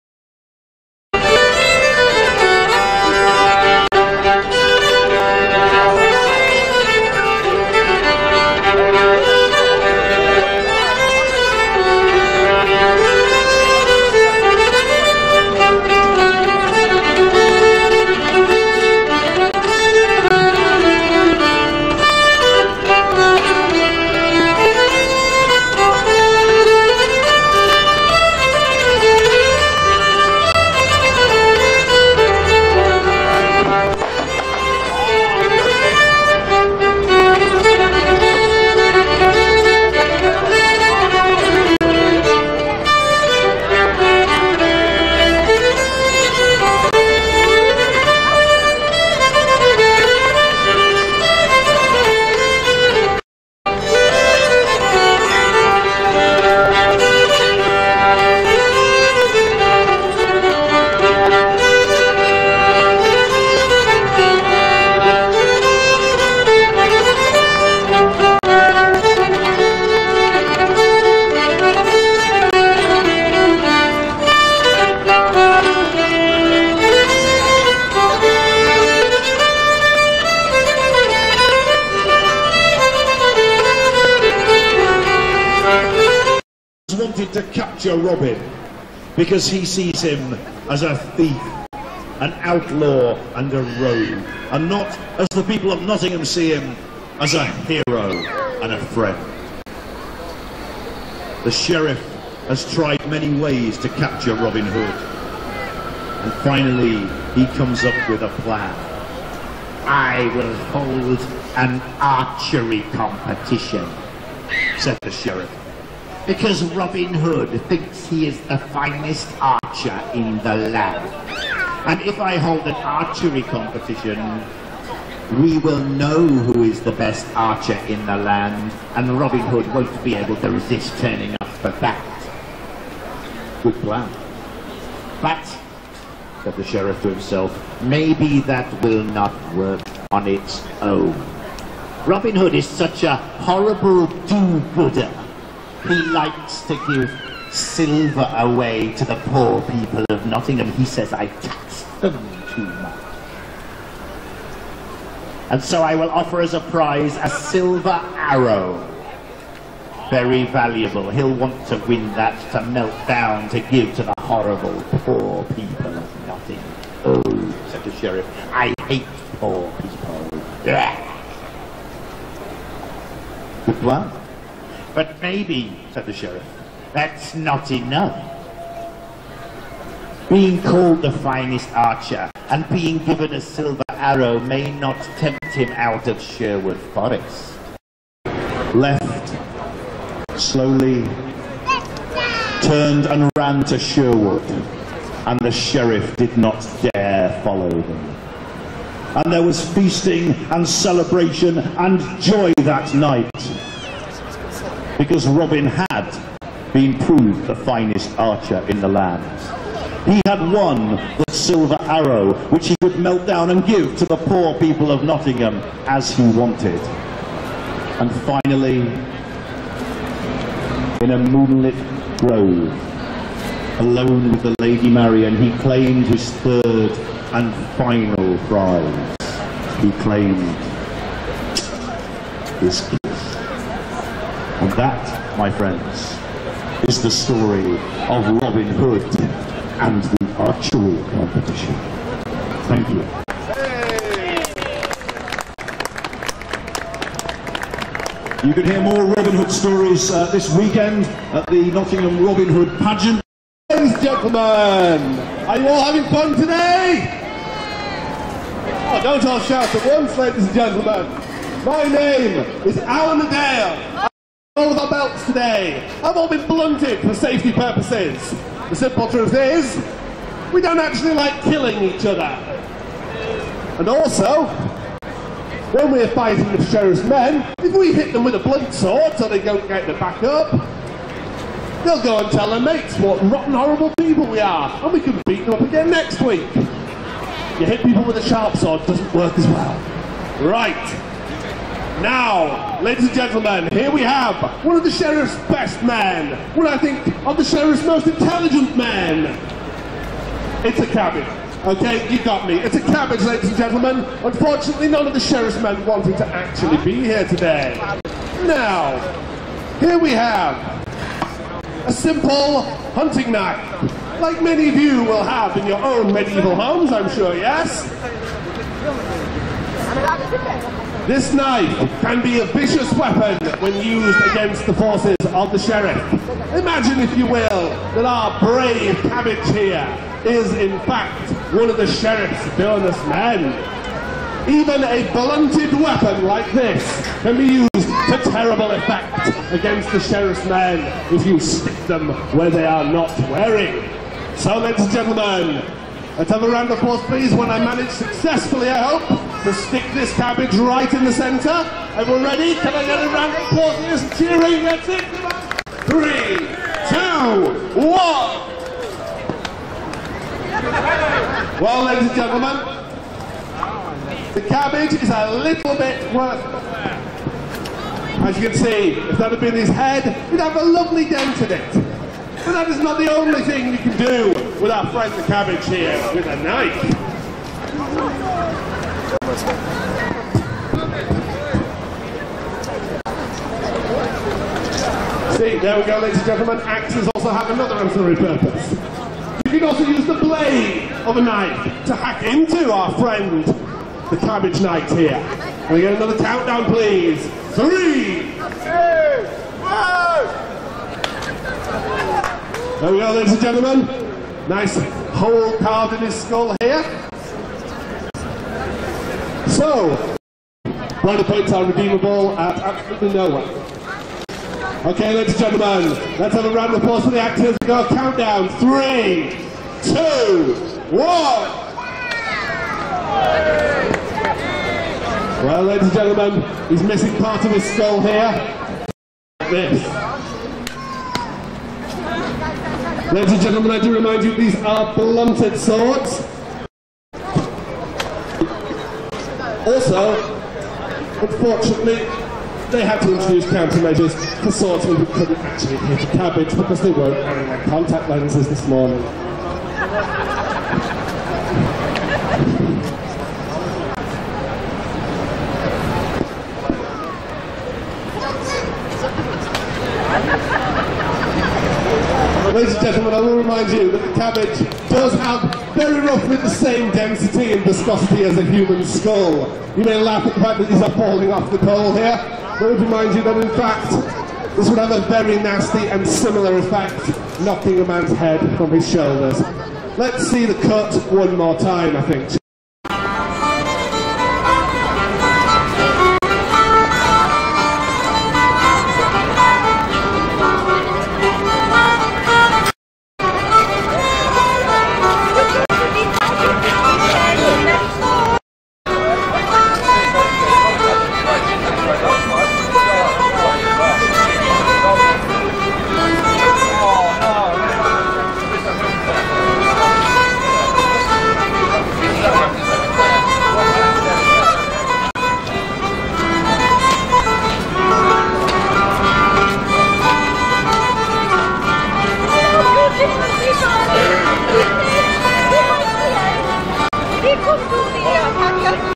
Robin Hood in Nottingham Market Square
Ye old music, an abridged story and cabbage murder